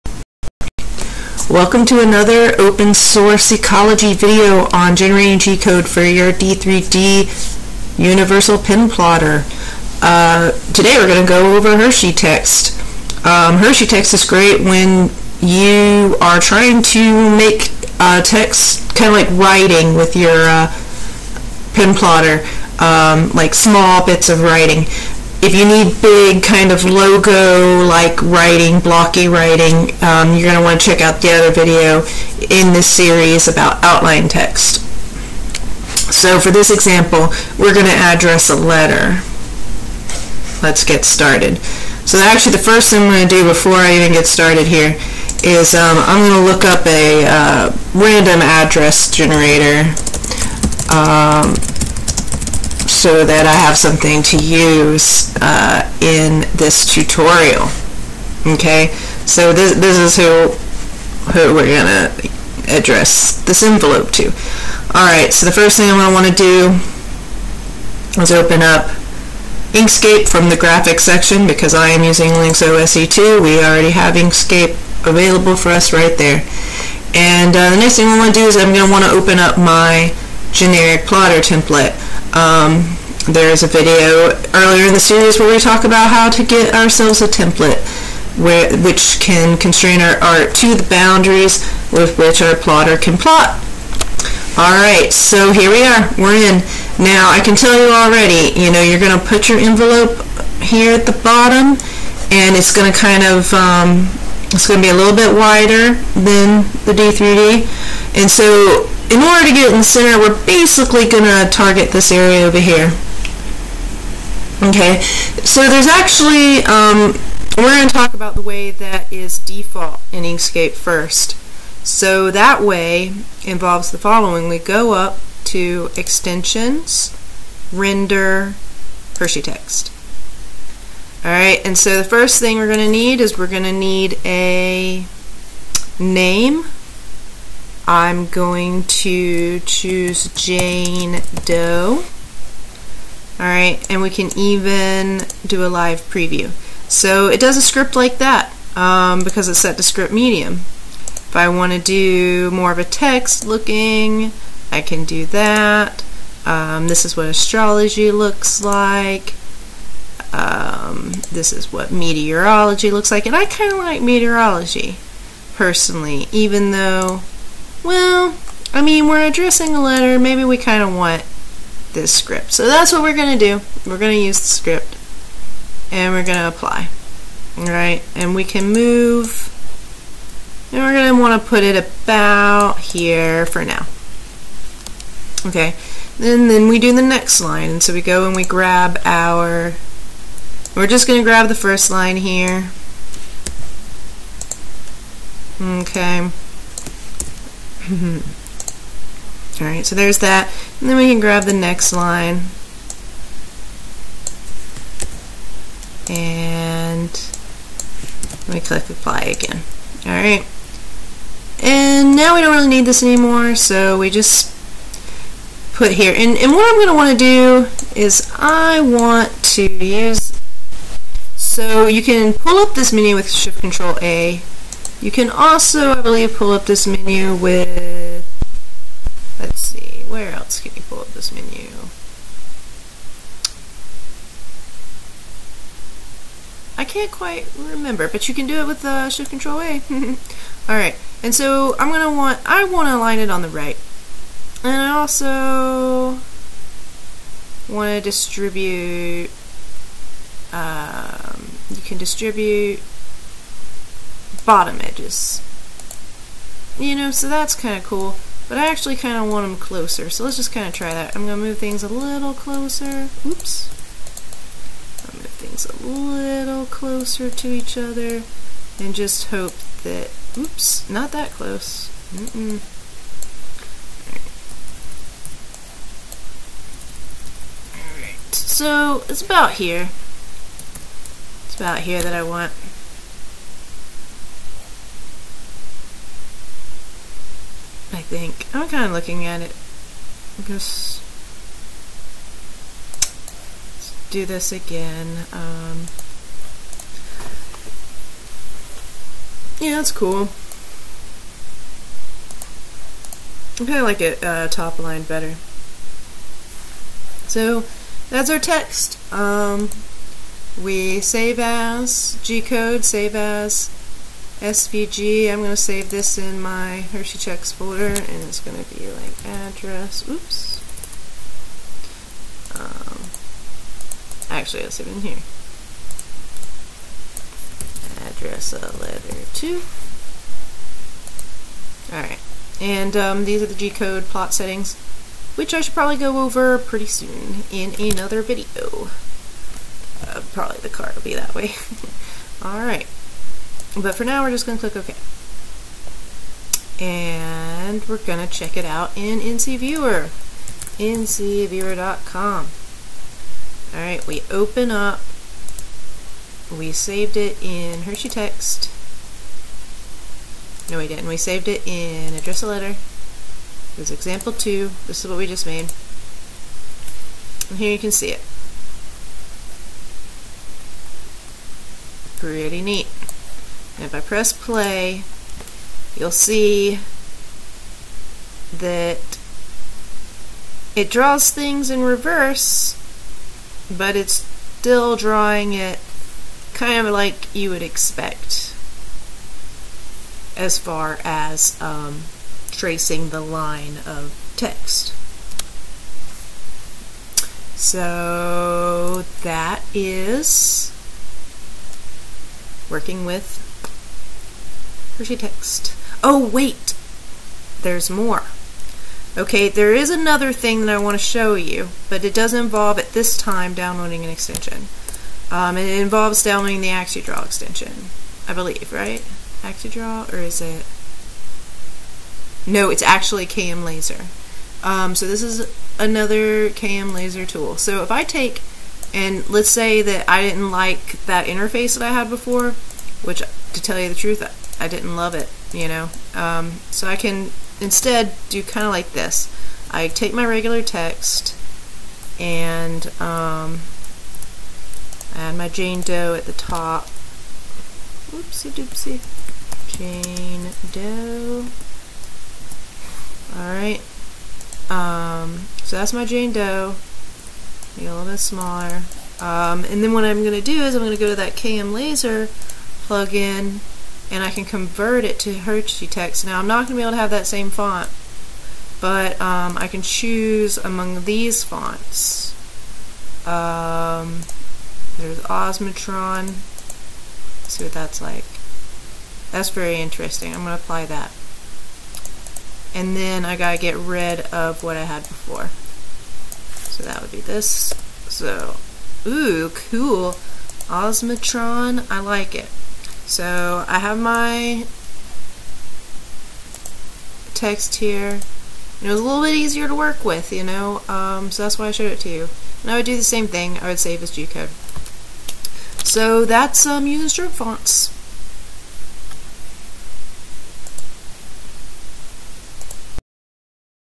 Stroke Font (Hershey Text)
Hershey text is what you want to have that "looks like writing" feel

For more blocky, graphical text, see the Outline Text video